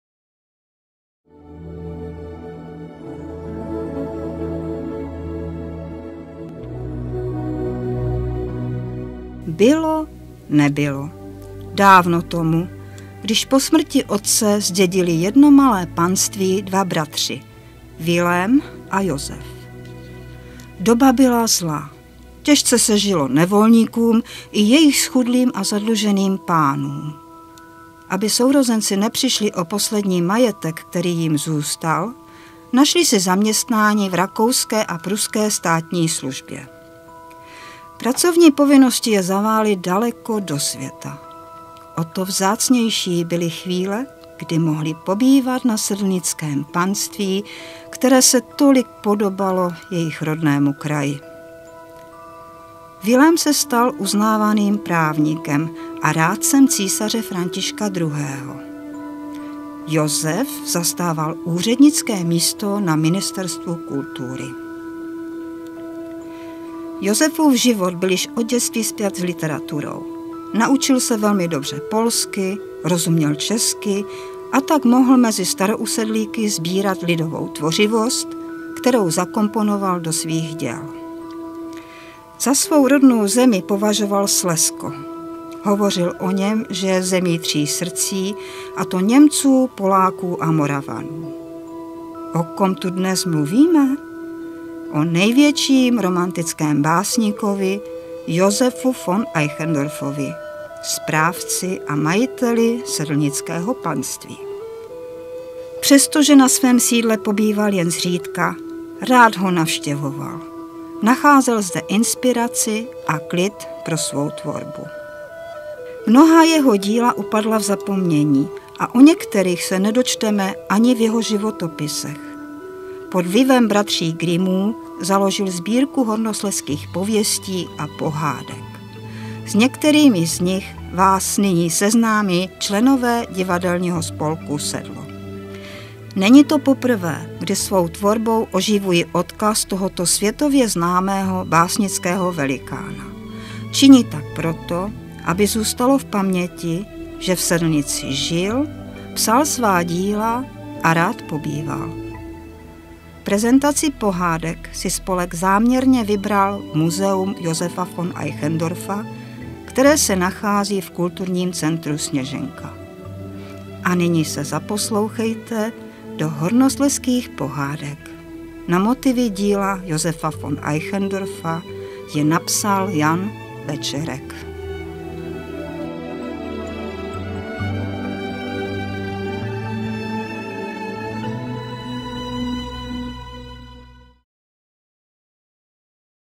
ZNĚLKA ČTENICE V MUZEU Josefa Von EICHENDORFFA
Úvod do Hornoslezských pohádek v podání členů divadelního spolku SEDLO Sedlnice